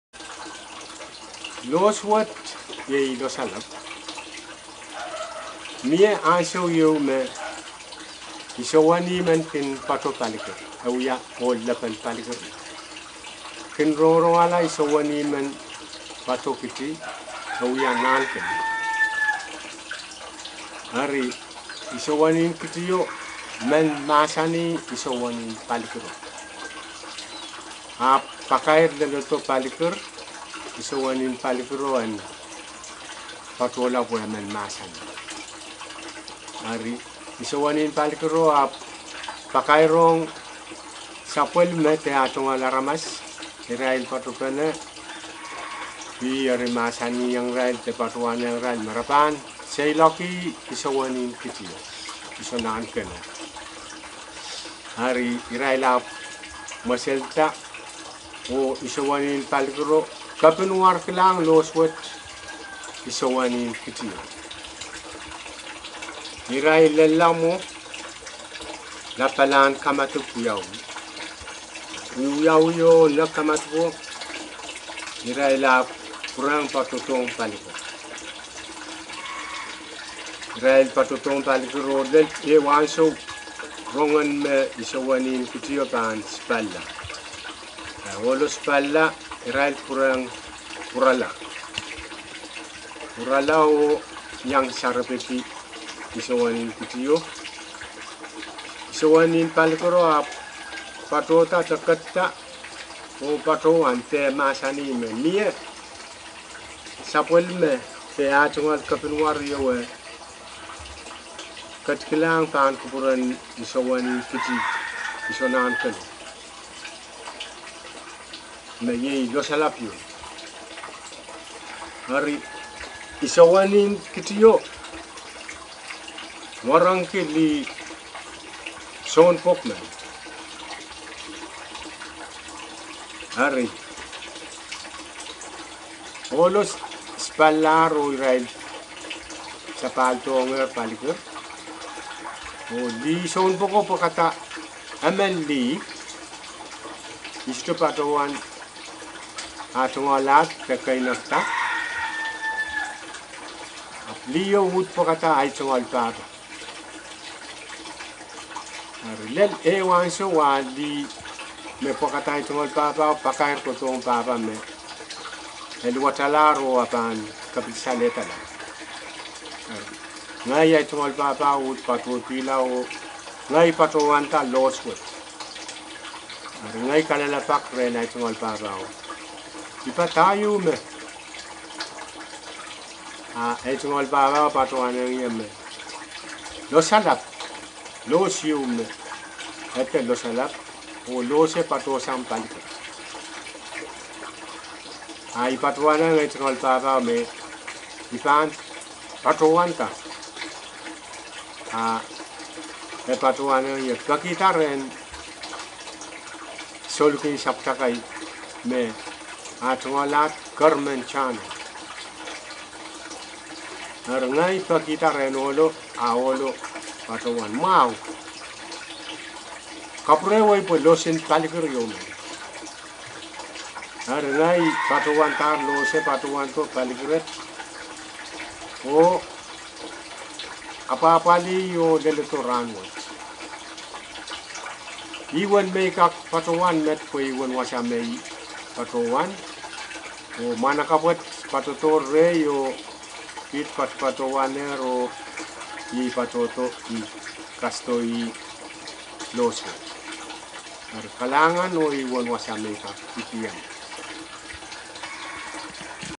Account of Lepen Palikir, Isoeni's Ceremonial Mat (Losalap), Pohnpei
His Royal Highness, Nahnken of Sokehs, Hilario Jack (b. 1953, 70 years old) narrates the account of Lepen Palikir, Isoeni’s ceremonial mat (losalap). The mat was a gift to Nahnken of Kitti, Nahnku (Isoeni), who was Nahnken from 1843 to 1864 and was the father of Henry Nanpei.

According to a historical record, Lepen Palikir was invited by Wasahi of Madolenihmw to join him at Rohnkiti in feasting Nahnken in 1864. Although he didn’t attend it, he invited Wasahi to Palikir. Three major chiefs, Lepen Palikir, Wasahi and Nahnken were political allies, who were in favor of American Protestant missionaries’ activities (Hanlon 1988: 120). There is no name recorded for this Lepen Palikir, although the paramount chief of Sokehs, Wasahi Sokehs, Keimw Sapwasapw, who seized power in Nett, taking the title of Lepen Nett until his death in 1874, also had a death honorific name (esenpweli), Isoeni (Hanlon 1988:153-154, 252).

This narration was documented by NGO Pasifika Renaissance (Executive Director Takuya Nagaoka) on March 12, 2023.

Reference: Hanlon, David, 1988. Upon a Stone Altar: A History of the Islands of Pohnpei to 1890. Honolulu : University of Hawaii Press.